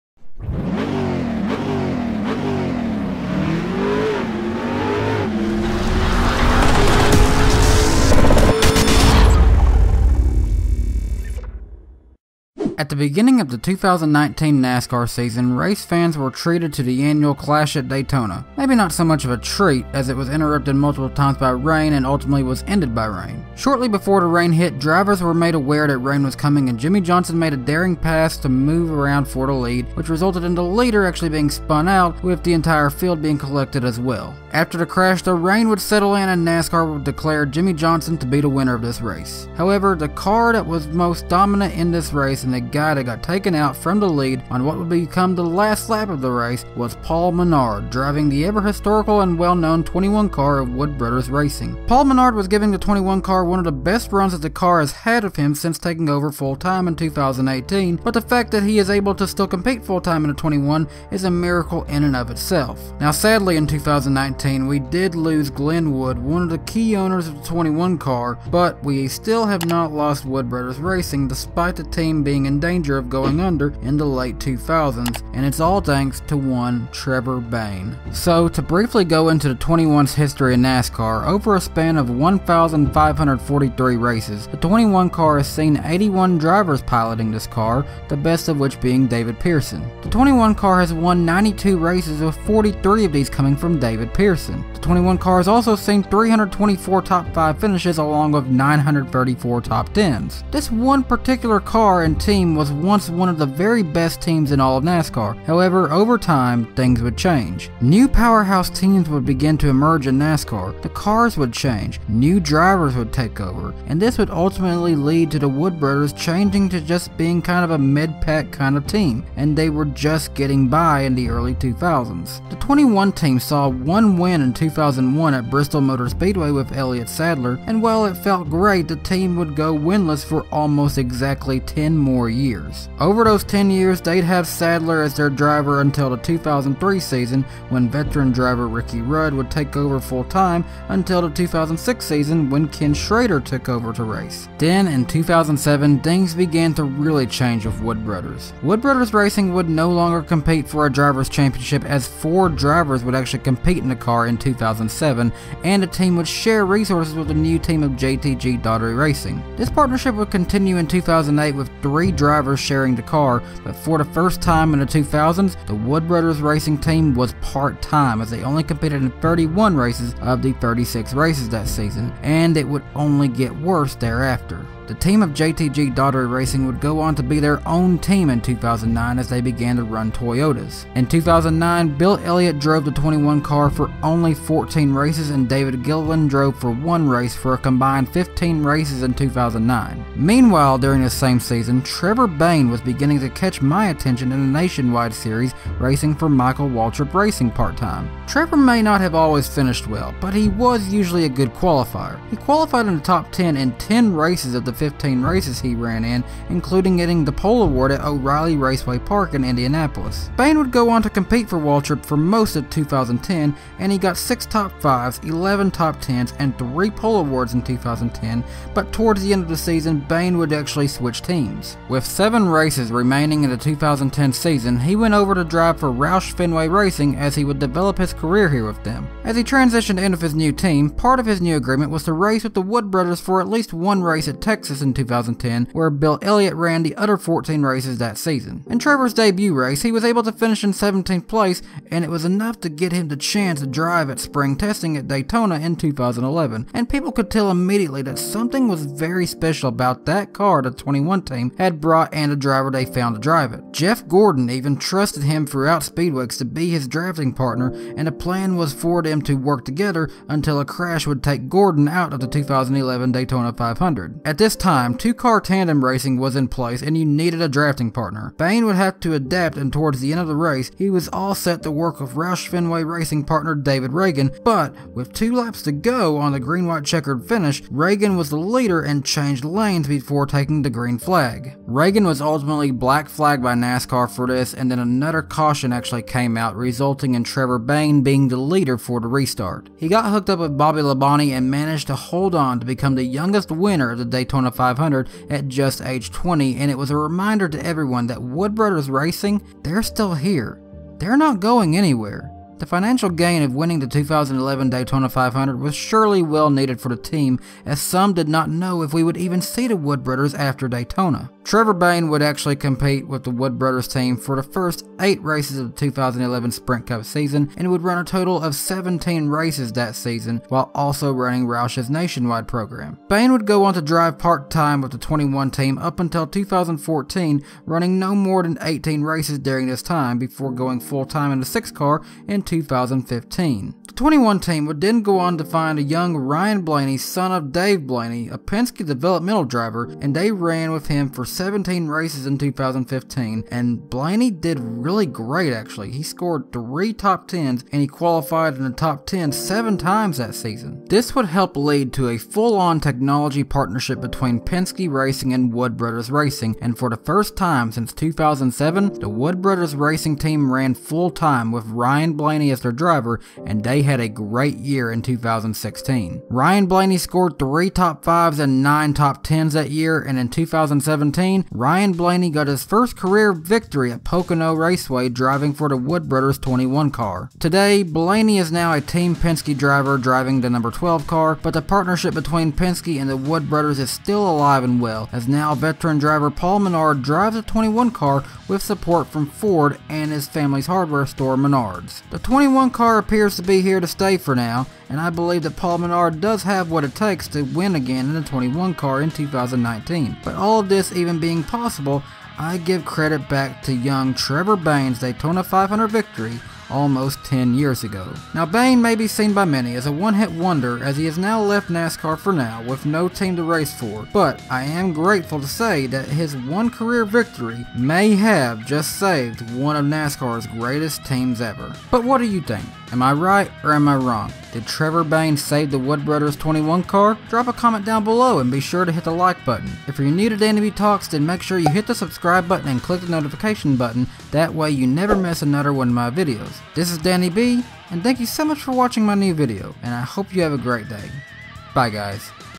Trevor Bayne: The Man Who Saved Wood Brothers Racing
Trevor Bayne may be seen by many as a one-hit wonder in NASCAR, but to me, I believe he is responsible for saving one of NASCAR's greatest teams ever, with his one and only win...here's why.

Who are some other drivers you would like to see get a full special video made on?